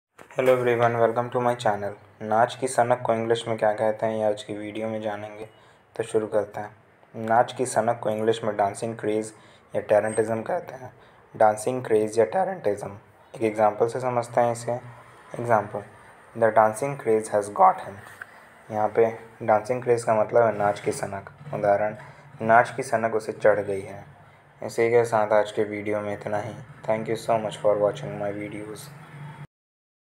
Naach ki sanak ko english mein kya kahate hain | नाच की सनक को इंग्लिश में क्या कहते हैं
Naach ki sanak ko English me kya kahate hain
Naach ki sanak को इंग्लिश में क्या कहते है

नाच‌‌ की‌ सनक को इंग्लिश में dancing craze कहते हैं
Naach ki sanak ko English mein dancing craze kahate hain